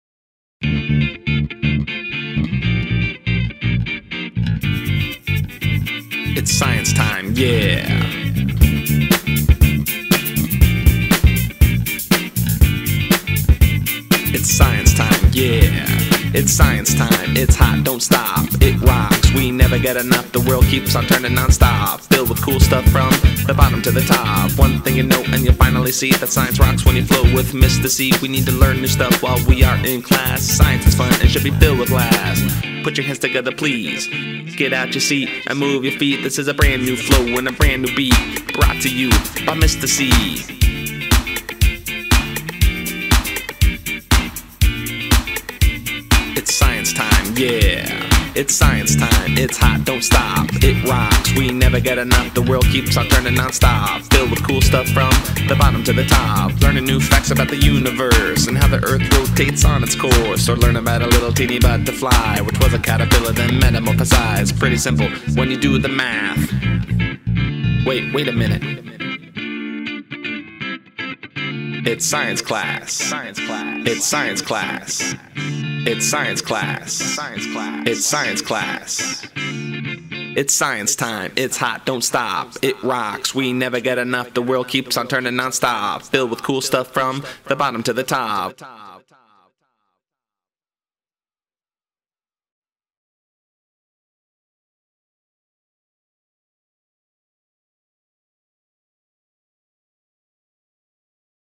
Boiling water & water gun in extreme -40 degrees cold (Southwest Ohio) | Mister C (Slow Mo #12)
Woke up to a crisp -10 degrees Fahrenheit this morning in Southwest Ohio. Thought I'd share what happens when you mix boiling water and a water gun and take it outside for a few shots.